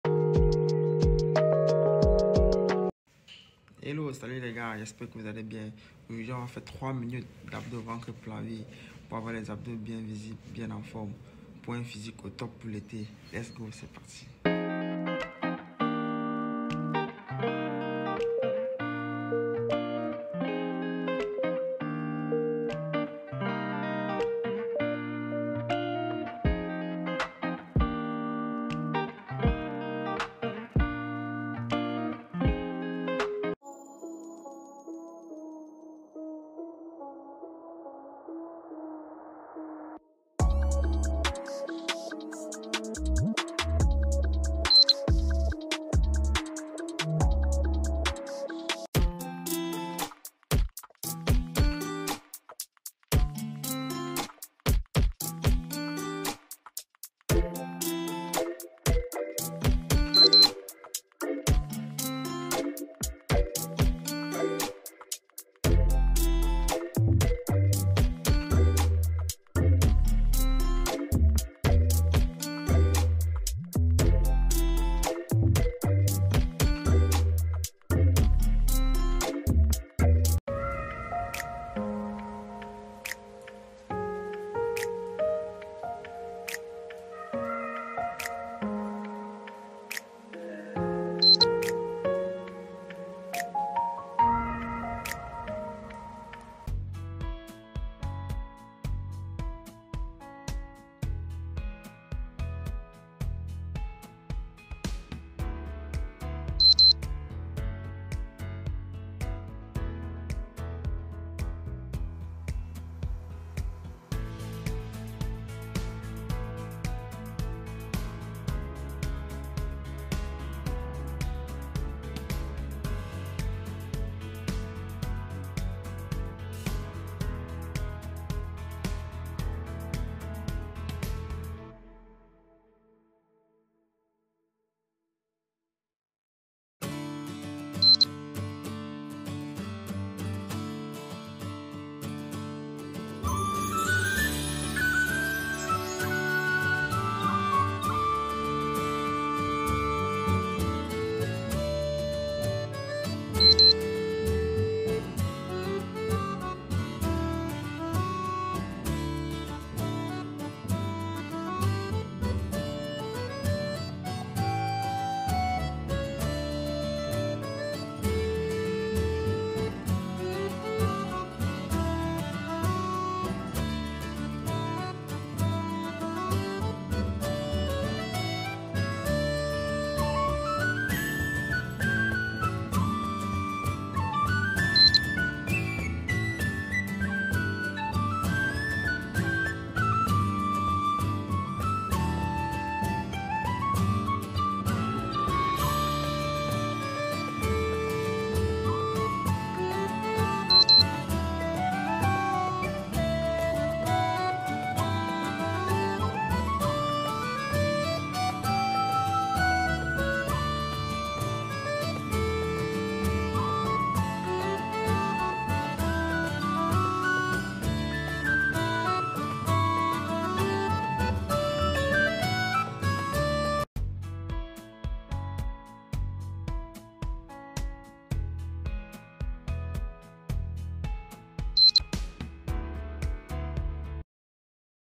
Comment avoir un ventre plat et des abdos visible???
Dans cette vidéo je vous présentes des exercices pour avoir un ventre plat et des abdos visible en seulement 2 semaines .. bon visionnage 👍🏽